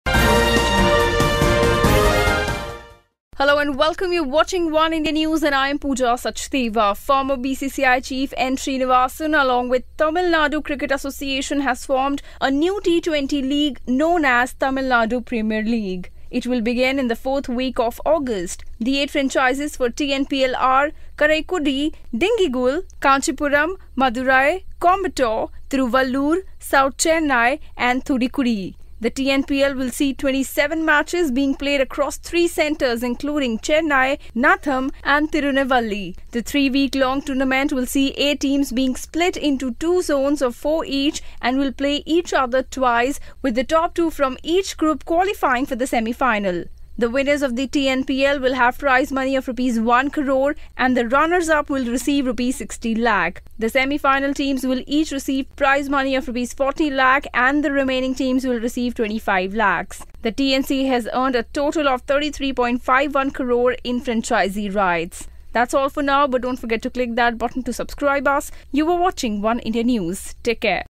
Tamil Nadu Premier League announced by ex-BCCI chief N Srinivasan | Oneindia News
Former BCCI chief N. Srinivasan along with Tamil Nadu Cricket Association has formed a new T20 league in Tamil Nadu known as Tamil Nadu Premier League. It will begin in the fourth week of August. The eight franchises for TNPL are Karaikudi, Dindigul, Kanchipuram, Madurai, Coimbatore, Tiruvallur, South Chennai, Thoothukudi. The TNPL will see 27 matches being played across three centres including Chennai, Natham and Tirunelveli